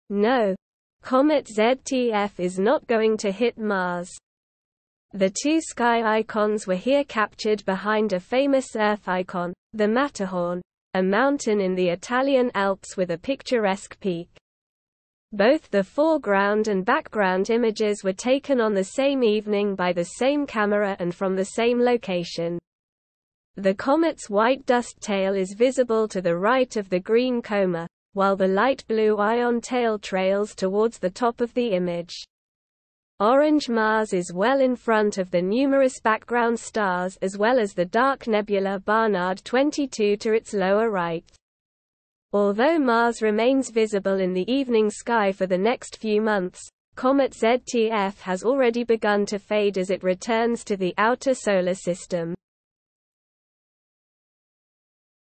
Comet ZTF and Mars
No, Comet ZTF is not going to hit Mars. Nicknamed the Green Comet for its bright green coma, C/2022 E3 (ZTF) did, however, pass almost in front of the much-more distant planet a few days ago, very near in time to when the featured picture was taken. The two sky icons were here captured behind a famous Earth icon -- the Matterhorn, a mountain in the Italian Alps with a picturesque peak. Both the foreground and background images were taken on the same evening by the same camera and from the same location. The comet's  white dust tail is visible to the right of the green coma, while the light blue ion tail trails towards the top of the image. Orange Mars is well in front of the numerous background stars as well as the dark nebula Barnard 22 to its lower right. Although Mars remains visible in the evening sky for the next few months, Comet ZTF has already begun to fade as it returns to the outer Solar System.    Comet ZTF Gallery: Notable Submissions to APOD

Image Credit & Copyright: Donato Lioce